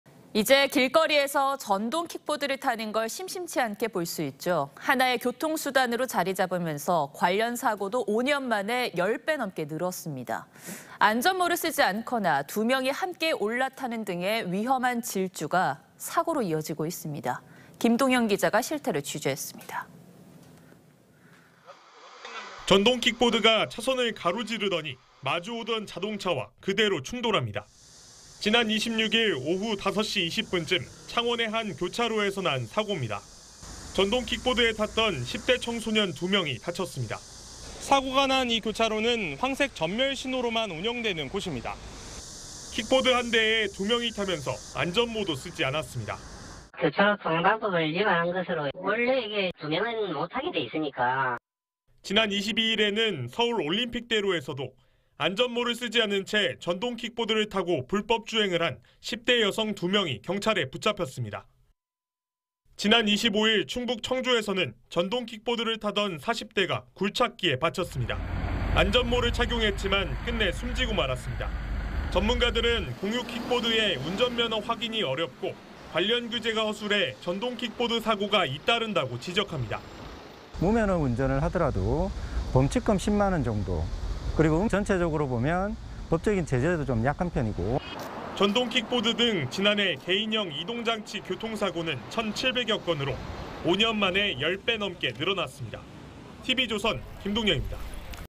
안전모 없이 도로 휘젓는 전동 킥보드…5년새 교통사고 10배
이제 길거리에서 전동 킥보드를 타는 걸, 심심치 않게 볼 수 있죠. 하나의 교통 수단으로 자리 잡으면서 관련 사고도 5년 만에 10배 넘게 늘었습니다. 안전모를 쓰지 않거나, 2명이 함께 올라타는 등의 위험한 질주가 사고로 이어지고 있습니다.